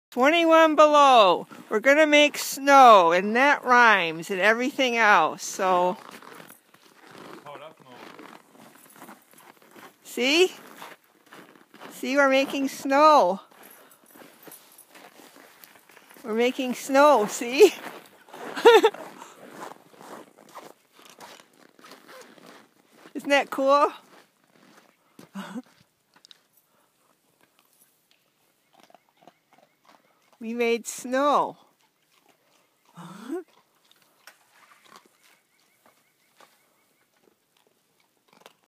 Science Experiment - Making Snow
Watching what happens to a pot of boiling water at -21 F.

Thursday, 2/27/2015 in Mercer, WI.